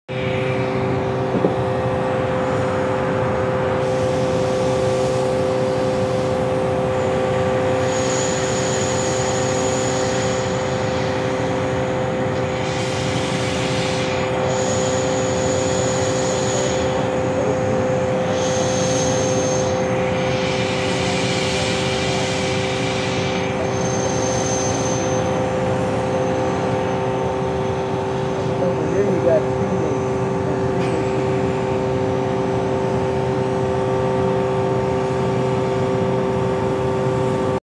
Acoustic Camera recording at a petrochemical plant
As shown, when the acoustic camera is scanned towards air compressors, where a high frequency whistle (at about 6000 Hz) can be heard.  From about a quarter mile away, the acoustic camera can “hear” steam leaks from pipes at the nearby refinery, and differentiate that noise from other equipment.  The acoustic camera enables us to identify the specific sources of noise and the frequency ranges of these noises so that optimum and cost-effective noise attenuation solutions can be developed.